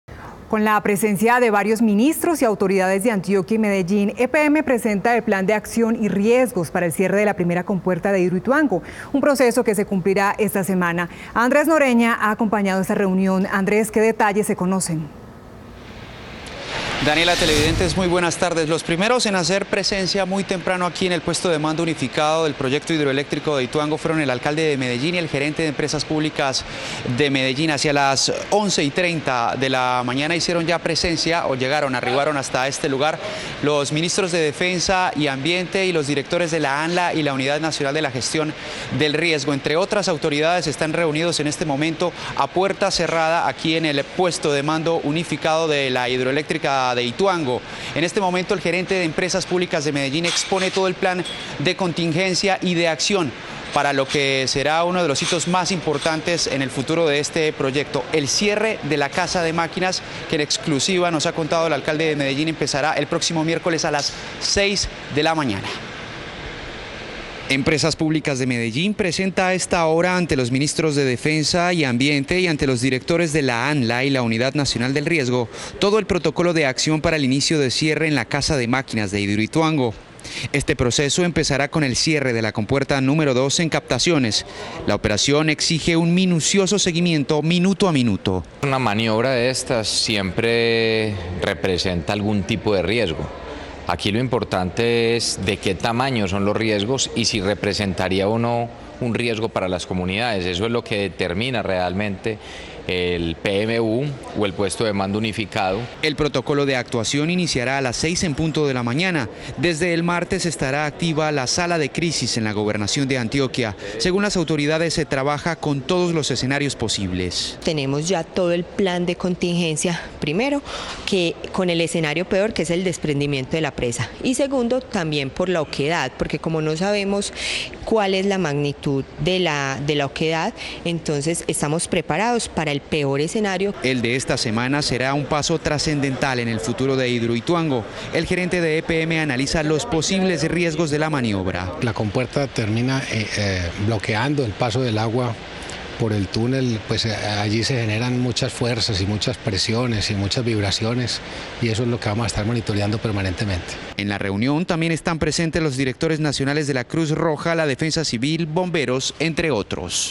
Definen protocolo de seguridad en Hidroituango por cierre compuerta en casa de máquinas
EPM presenta este domingo ante ministros, directores de la ANLA y la Unidad Nacional de Gestión del Riesgo las acciones para este procedimiento.